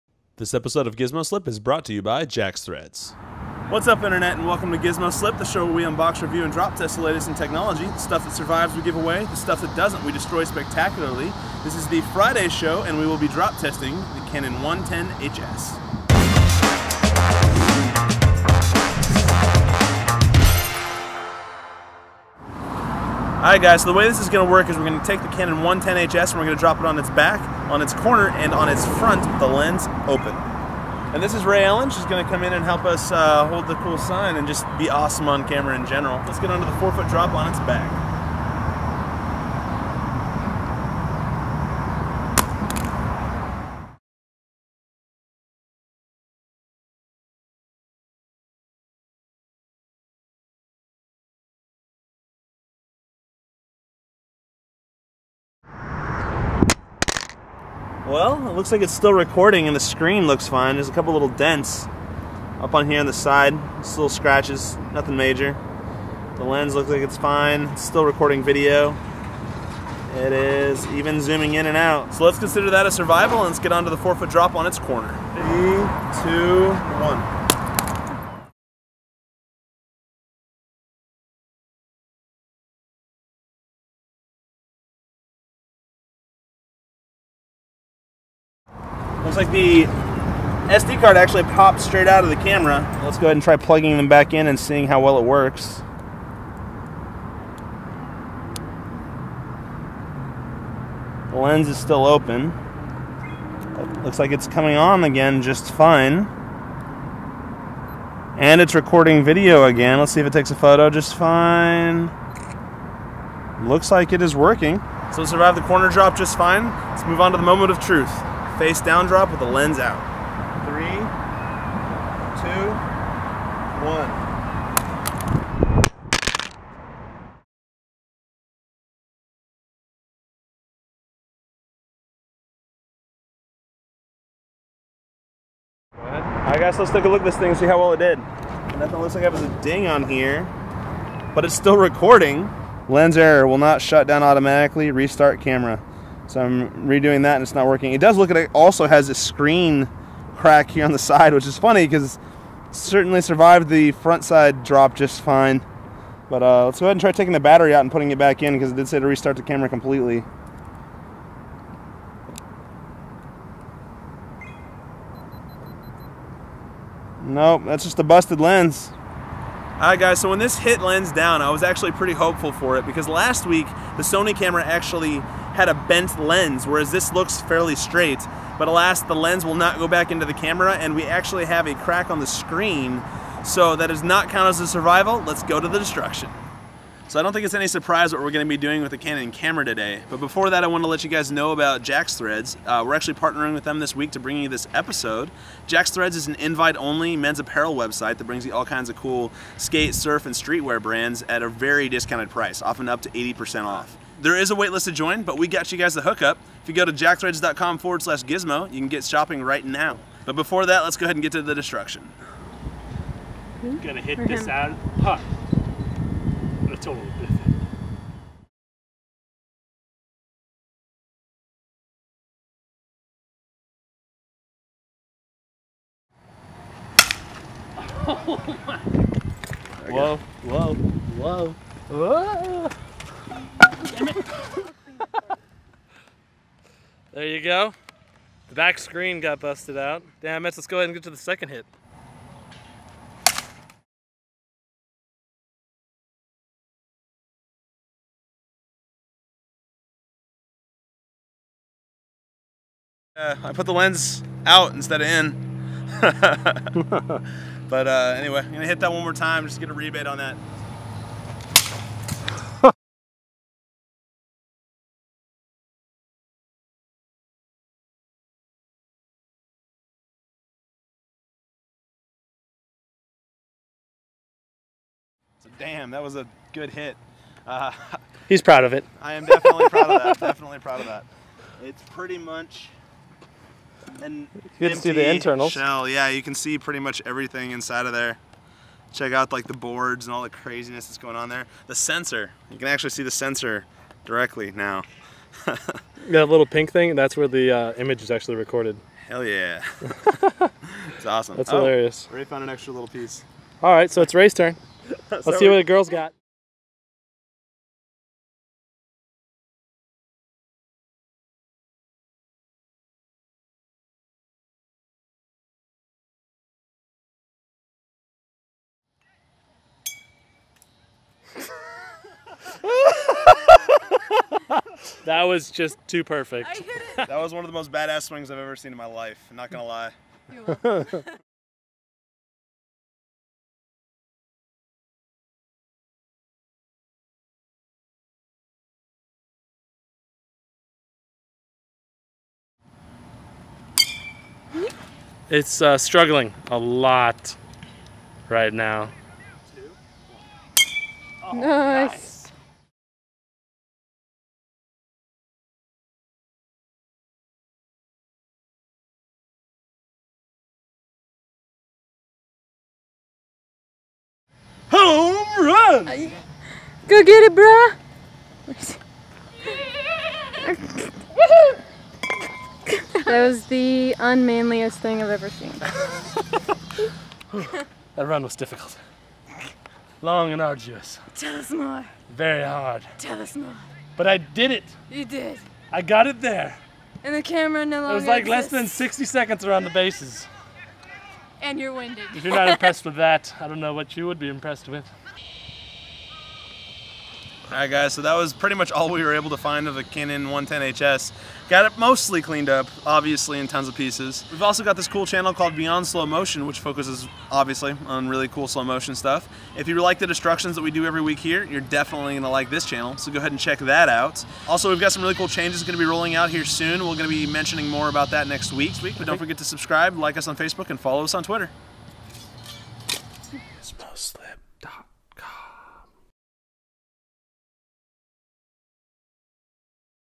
Drop Test: Canon ELPH 110 HS
The Canon ELPH 110 HS is an excellent camera, but how durable is it? Watch and find out!